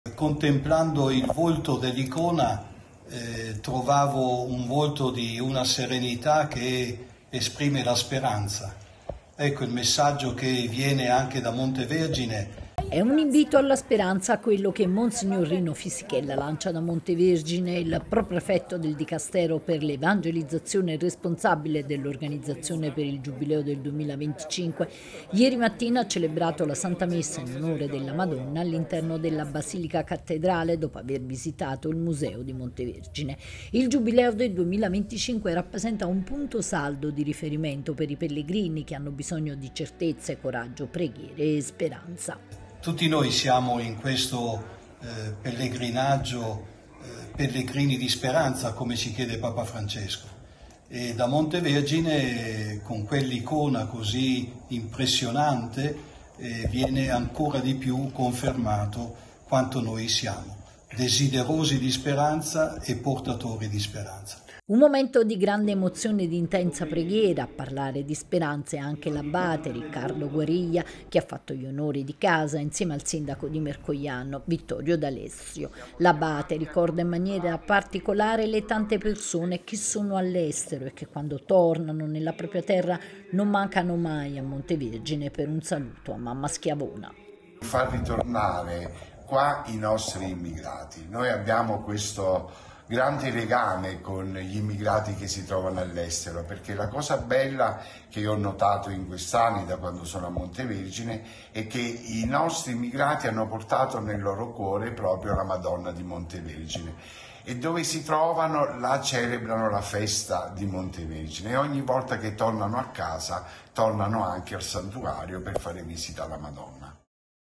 Da Montevergine il messaggio di speranza per il Giubileo
Intensa giornata a Montevergine per Monsignor Fisichella, organizzatore del Giubileo 2025. Il santuario si appresta ad essere protagonista. L’Abate Guariglia: “L’occasione per tanti emigrati di tornare”.